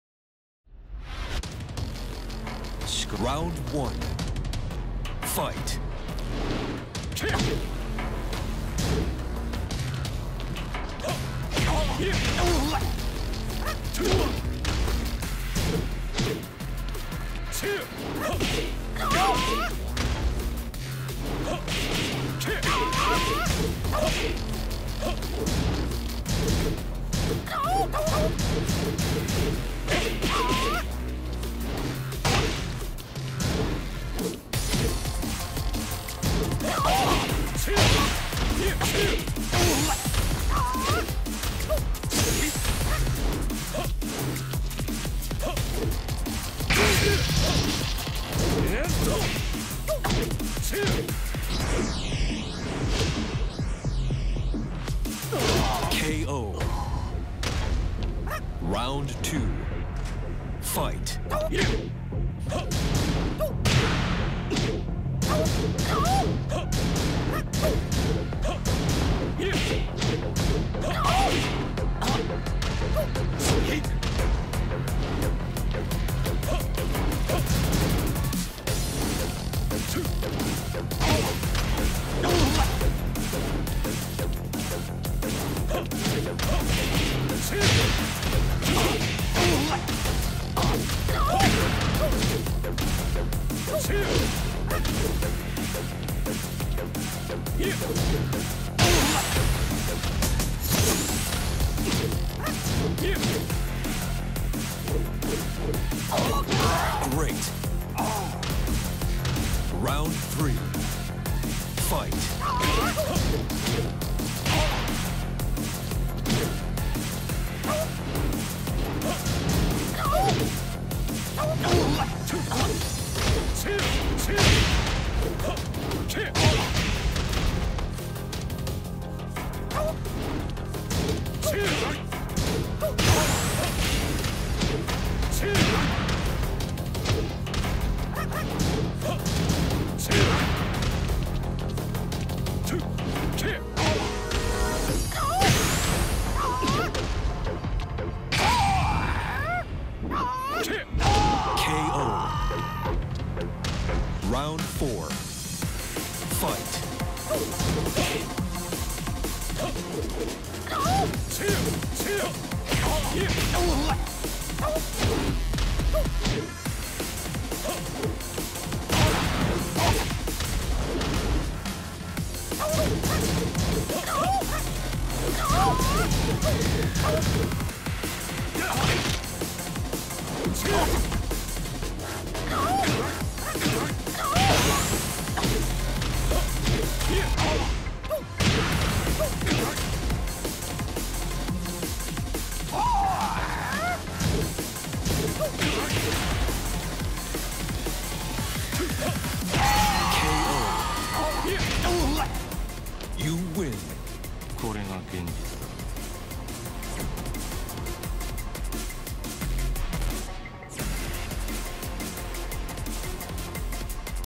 I finaly made it to Grand Master with Jin!
I've been learning Jin for 2 weeks now and finaly made it to grand master.